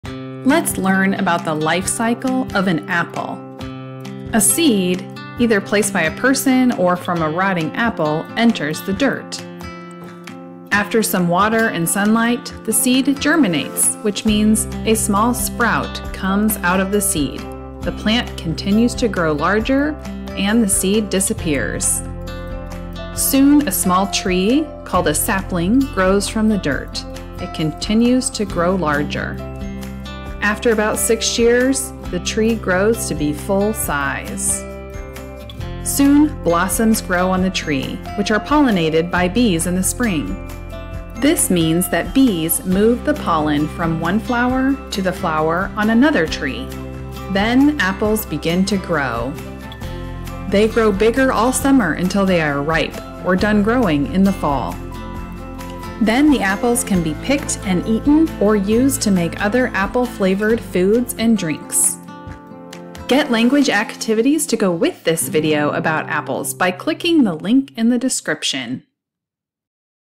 Apple Life Cycle Lesson for Kids
Learn about how an apple goes from seed to fruit with this video showing an apple's life cycle. Get no prep activities to use with this lesson: